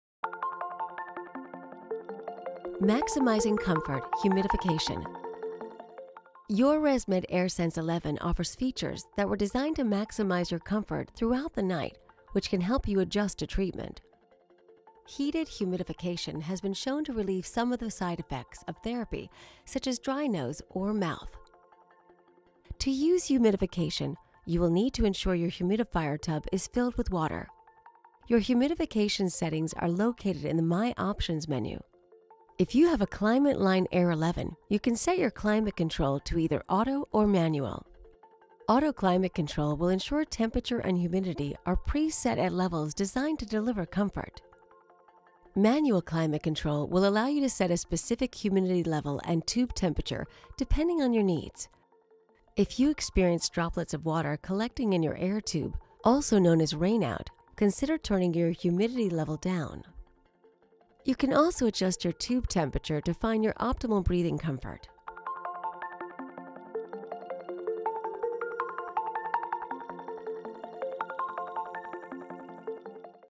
ResMed AirSense 11: Maximize your comfort with humidification
ResMed AirSense 11 features a heated humidification option to help maximize your comfort and prevent a dry nose or mouth. Use the humidification settings to adjust temperature and humidity to optimal levels for your individual breathing comfort.